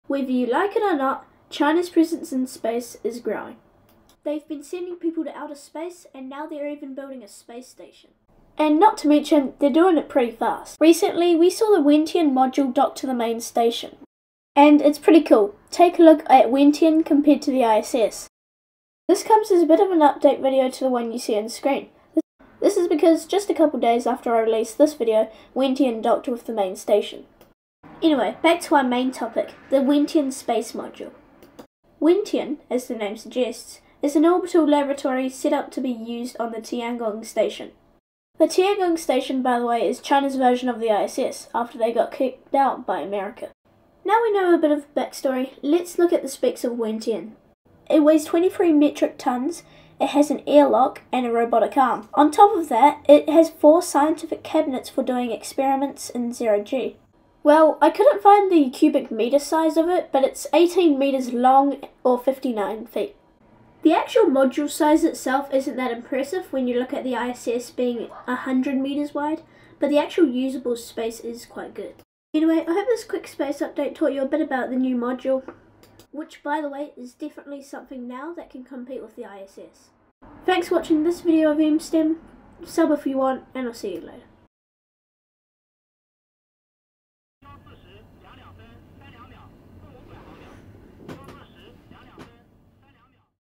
Wentian Module - China's Scientific Space Powerhouse
In this one I take a look at the newly - added wentian space module. This was added to the Tianhe space module to start construction on China's space station, called Tiangong. O

fficially the Wentian laboratory cabin module, is a major module of the Tiangong space station. It is the first Laboratory Cabin Module launched,

and the first module to extend the existing Tianhe core module of the station.
- Wikipedia

This is a follow - up video to my "Tiangong - China's Space Monopoly" video.
Sub if u want
pls